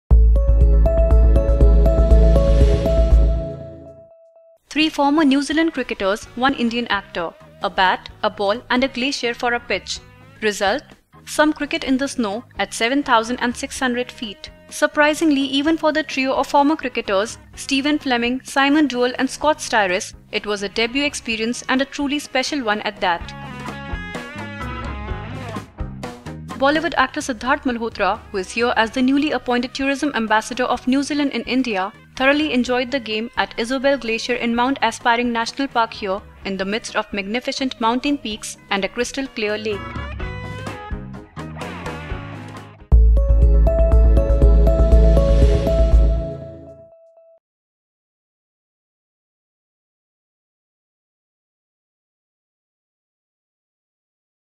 Sidharth Malhotra plays cricket in snow with Steven Fleming, Simon Doull and Scott Styris
Three former New Zealand cricketers, one Indian actor. A bat, a ball and a glacier for a pitch. Result? Some cricket in the snow at 7,600 feet!
Surprisingly, even for the trio of former cricketers -- Steven Fleming, Simon Doull and Scott Styris, it was a debut experience and a truly special one at that. Bollywood actor Sidharth Malhotra, who is here as the newly appointed tourism ambassador of New Zealand in India, thoroughly enjoyed the game at Isobel Glacier in Mount Aspiring National Park here in the midst of magnificent mountain peaks and a crystal clear lake.